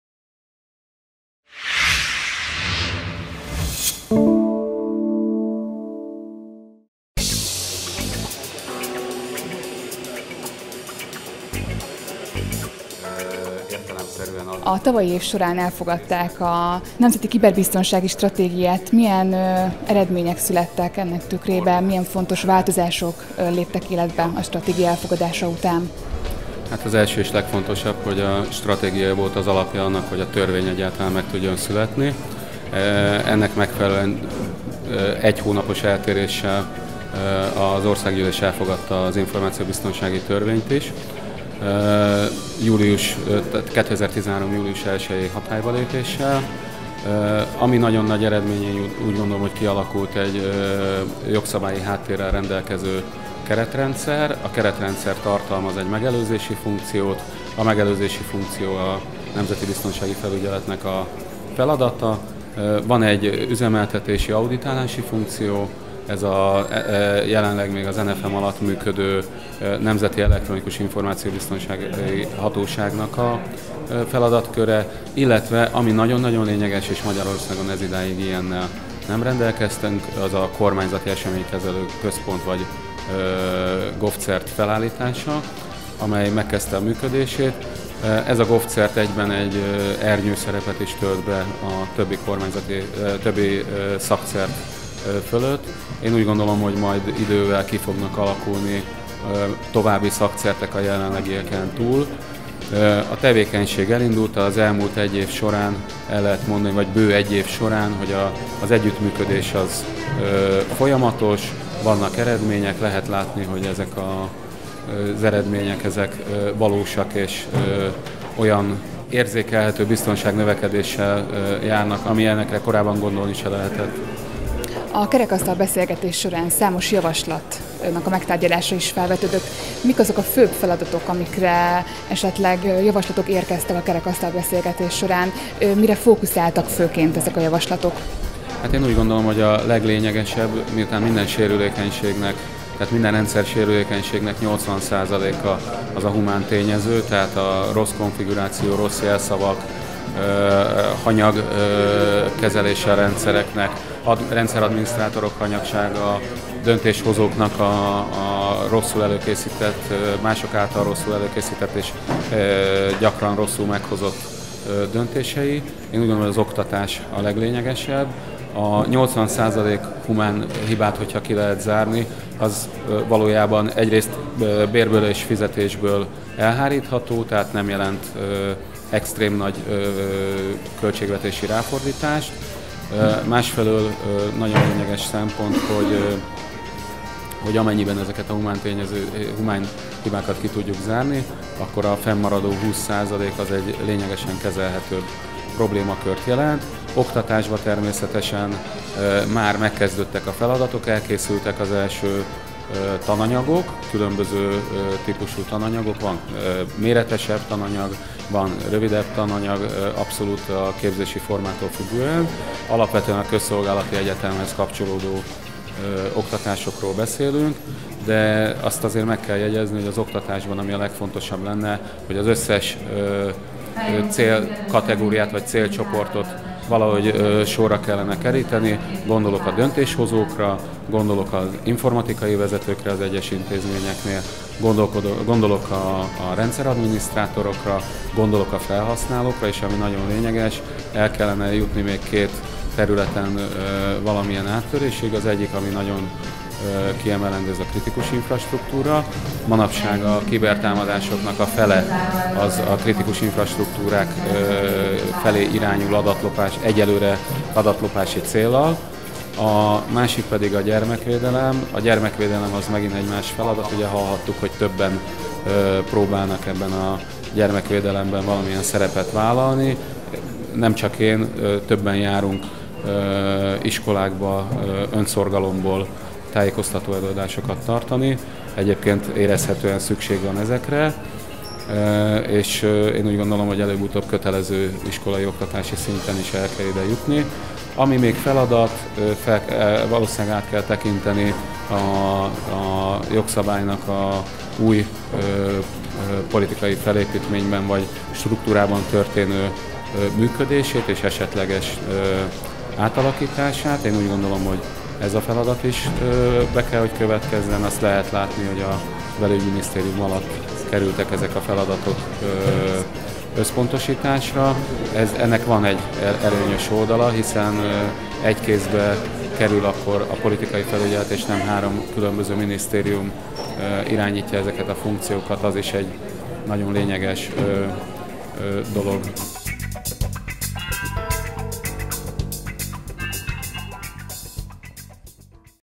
A Nemzeti Kiberbiztonsági Stratégia célja és eredményei - interjú Zala Mihállyal
A Nemzeti Kiberbiztonsági Stratégia volt az alapja az információbiztonsági törvény megalkotásának. A stratégia és a törvény részleteiről Zala Mihály beszélt az Infotér Konferencián. A Nemzeti Biztonsági Felügyelet elnöke elmondta, a stratégiával létrejött egy jogszabályi háttérrel rendelkező keretrendszer, ami tartalmaz egy megelőzési funkciót, egy üzemeltetési- auditálási funkciót és ennek köszönhetően jött létre a Kormányzati Eseménykezelő Központ is.